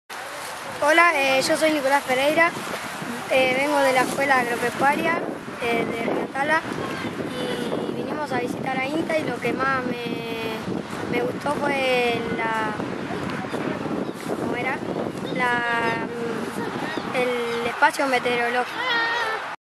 Nicolás Pereyra Escuela Agropecuaria Vuelta de Obligado
Participó de la Vidriera tecnológica 2016 y dejó su testimonio. Gracias por venir!!